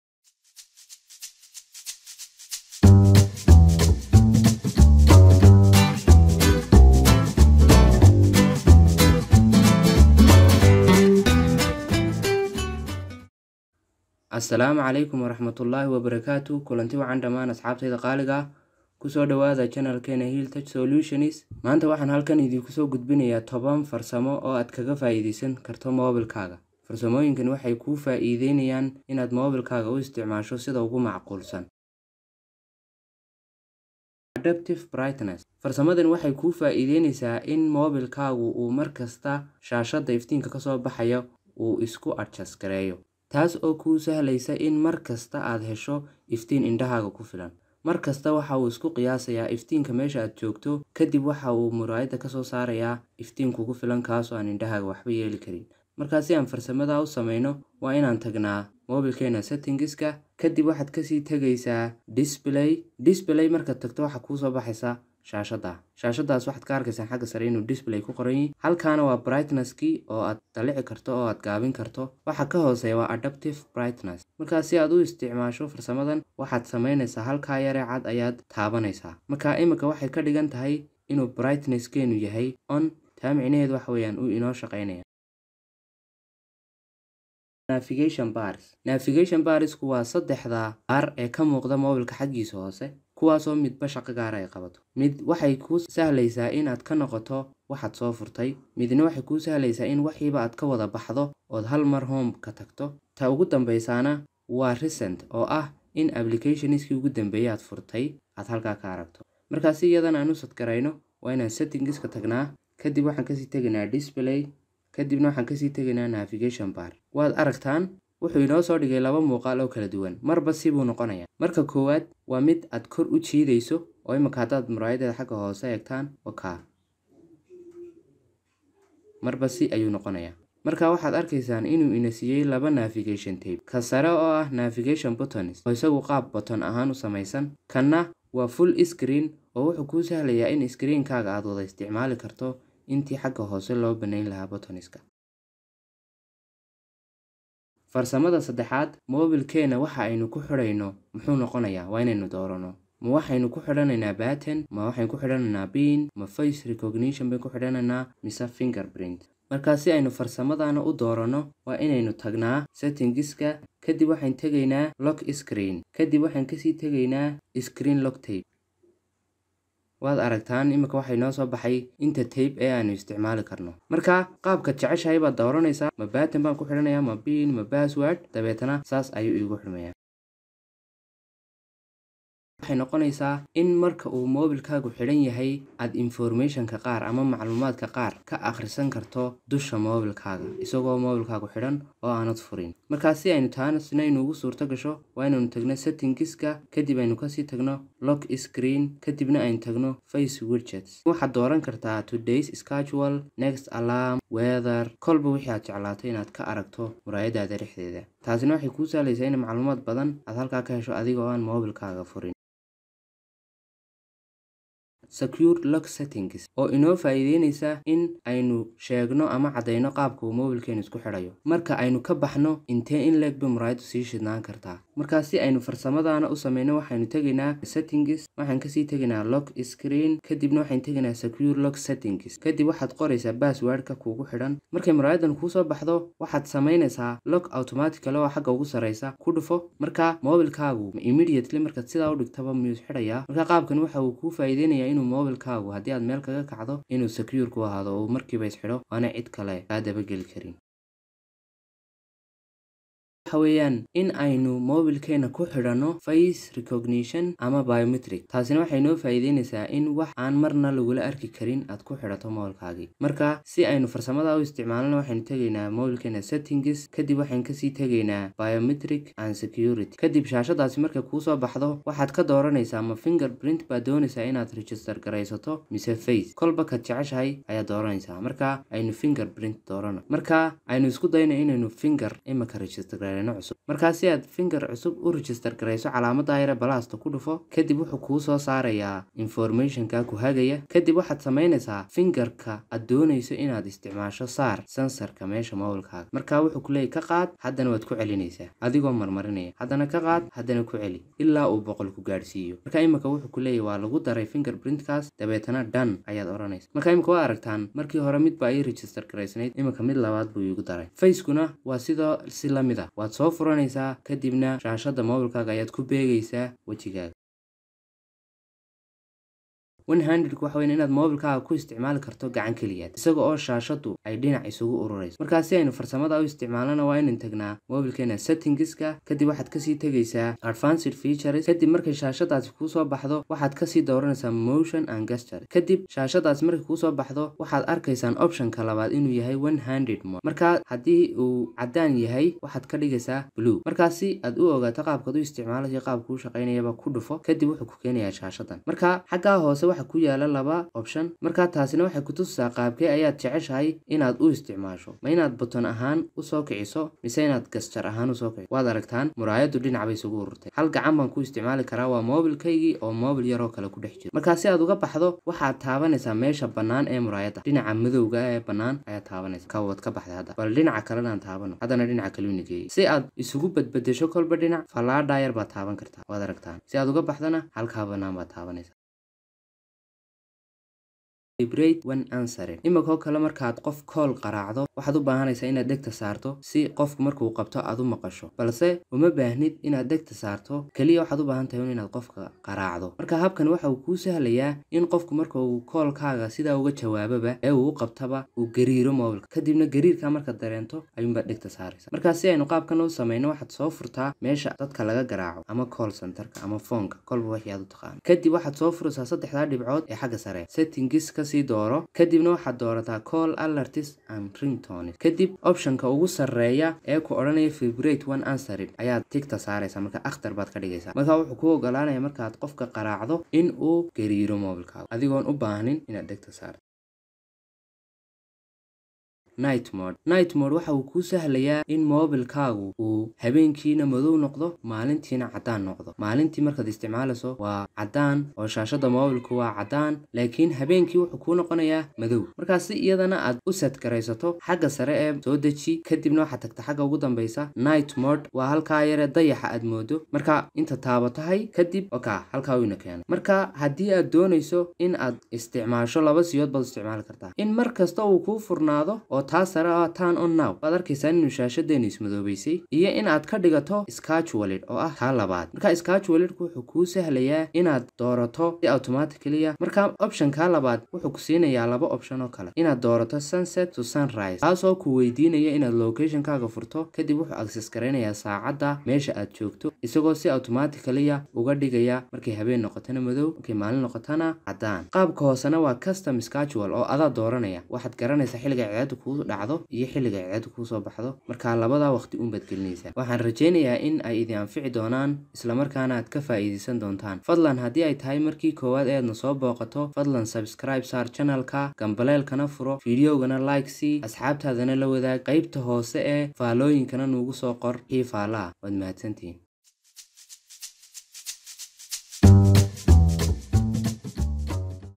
Ka baro 10 farsamo moobilkaaga | 10 Smart Features of your mobile phone | HiilTech Solutions
Videogan waxa aad ka faaiidaysaa farsamooyin uu leeyahay moobilkaagu kuwaas oo aad adeegsan karto, si ay kor ugu qaadaan isticmaalka mobilkaaga.